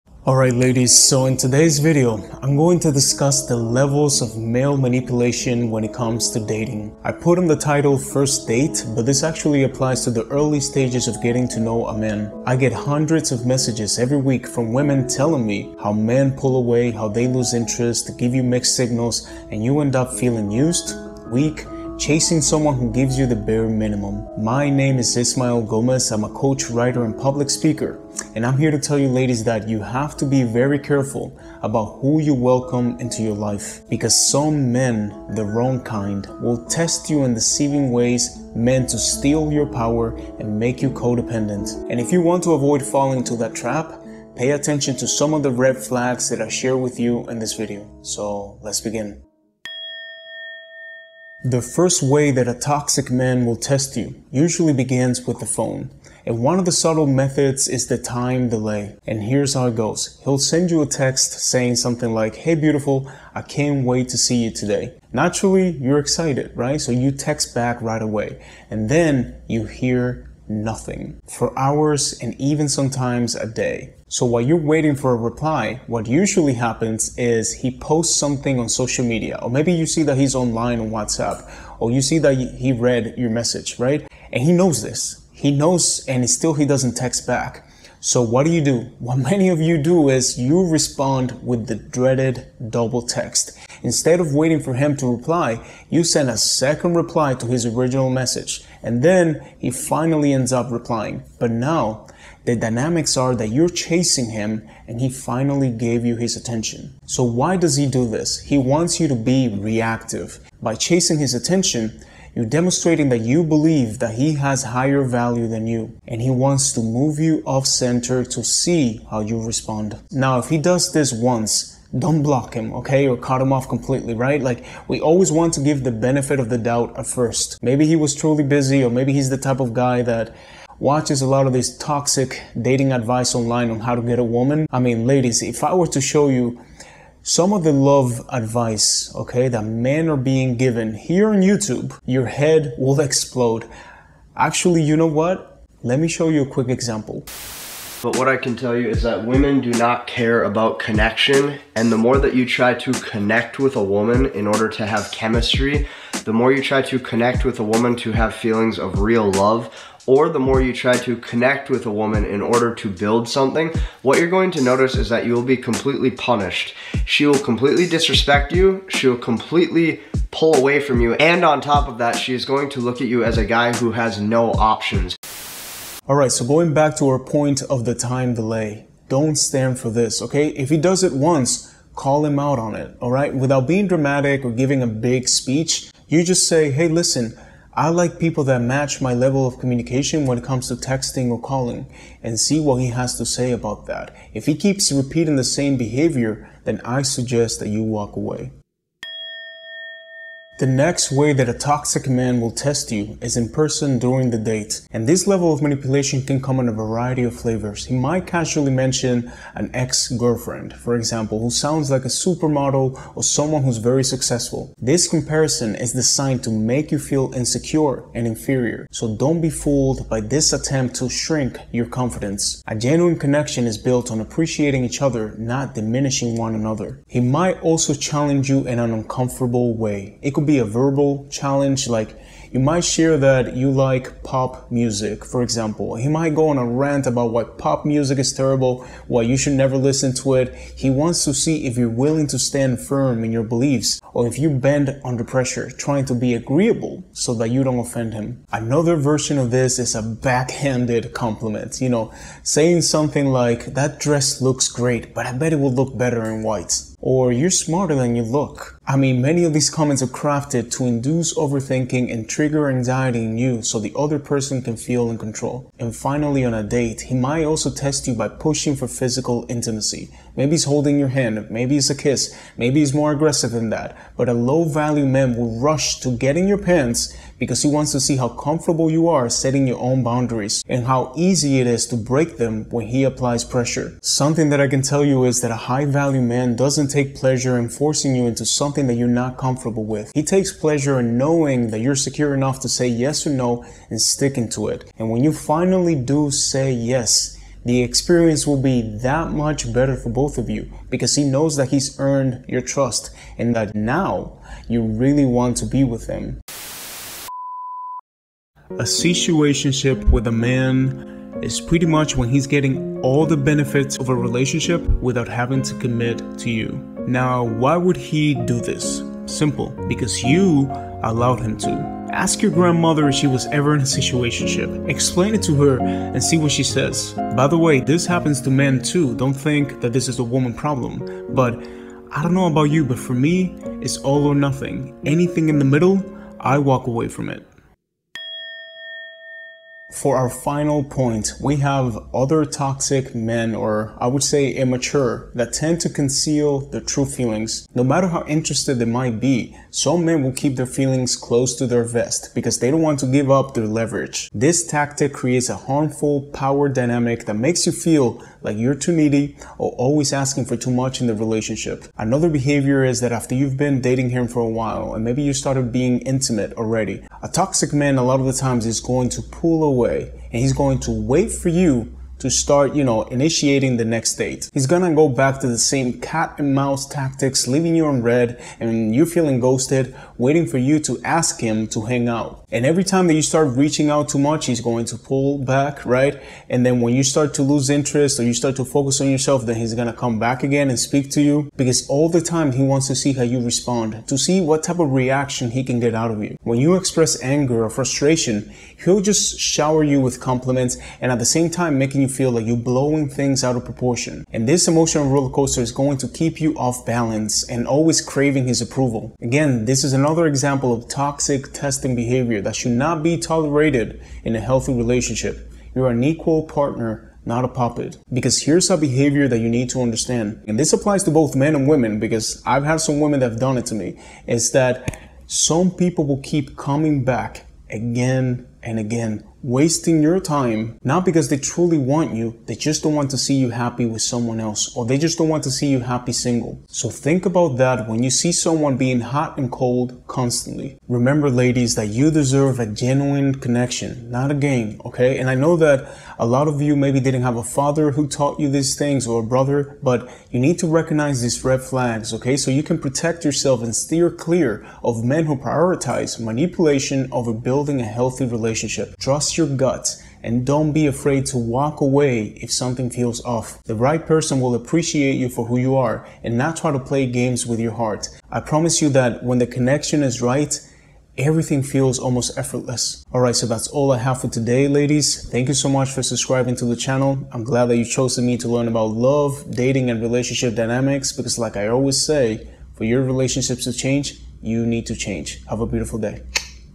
How Men Test You On A First Date - Watch Out For These Signs
In today's video, I will show how some Men Test You On A First Date. They will do this in deceptive ways to steal your power and make you feel dependent. So, to avoid falling into these traps, pay attention to the red flags I share with you.

CHAPTERS
00:00 Topic Summary
00:50 How Men Test You On A First Date 01
03:20 How Men Test You On A First Date 02
05:08 Quick Dating Advice For Women
05:39 How Men Test You On A First Date 03
07:21 Closing

01. The Time Delay
A subtle manipulation tactic often starts with text messages. He might send a text saying, "Hey, beautiful. Can't wait to see you!" You respond eagerly, but then... silence. Hours pass, maybe even a day, and you hear nothing.
Meanwhile, he's on social media, posting things he knows you'll see. This delay is a test to see if you'll chase his attention, demonstrating that you believe he holds a higher value. If he repeats this behavior, it's a clear sign he's trying to gain psychological leverage.

02. In-Person Tests
Moving on to in-person manipulation, he might casually mention an ex who sounds like a supermodel or is highly successful, aiming to make you feel insecure. Don't be fooled by these comparisons. He might also challenge your beliefs, like criticizing something you enjoy, to see if you'll stand firm or bend under pressure. Backhanded compliments are another tactic designed to induce overthinking and trigger anxiety, putting him in control. Lastly, he might push for physical intimacy on the date, testing your boundaries and comfort levels.

03. Concealed Feelings
Now, let's talk about men who conceal their true feelings. Some will hide their emotions to maintain leverage, creating a power dynamic where you feel needy or asking for too much. After a few dates and perhaps intimacy, he might wait for you to initiate the next date, playing a cat-and-mouse game. If you don't react to his pullback, he may return once he realizes you have other priorities, keeping you off-balance and craving his attention.

It's crucial to recognize these toxic behaviors and not tolerate them. In a healthy relationship, you're an equal partner, not a puppet. Some people will keep coming back not because they want you but because they don't want to see you happy with someone else. So, when someone plays hot and cold, consider their intentions.

Ismael Gomez III
I'm a Cuban - American Relationship Coach, Author, and Speaker.